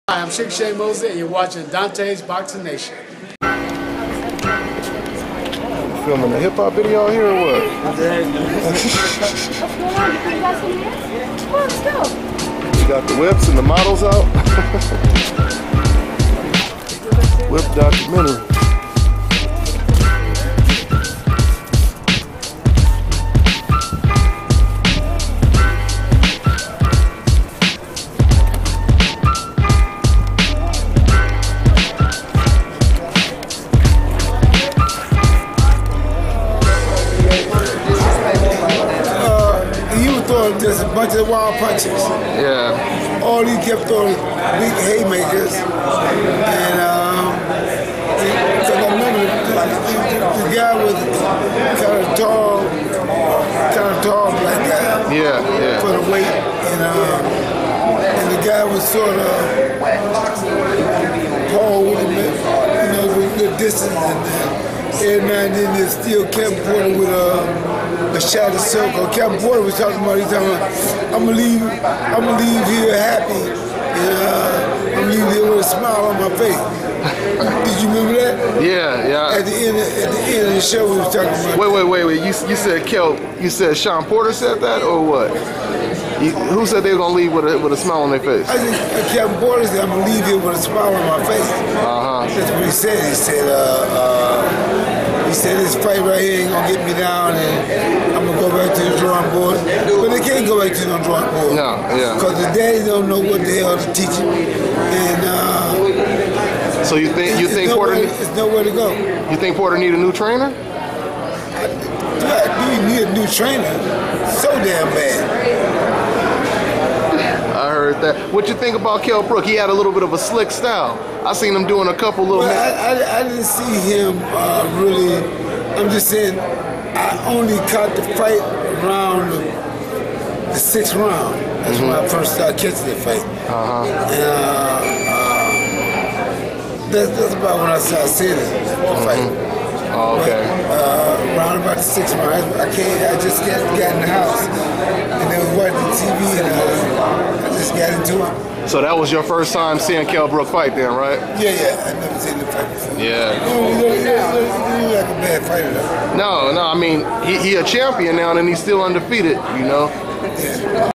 MAYWEATHER SR: "SHAWN PORTER NEEDS A NEW TRAINER"
(Exclusive interview) Following the Brook vs Porter fight I caught up with Floyd Mayweather Sr and here's what he had to say.